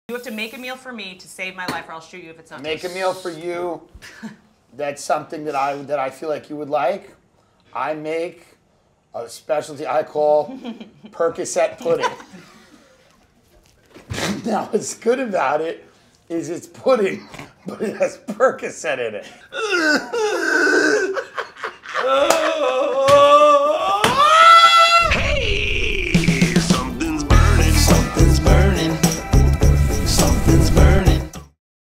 Tim Dillon Should Host A Cooking Show
@TimDillonShow should host a cooking show. Tag a comic who should come on SomethingsBurning and what dish Tim should make them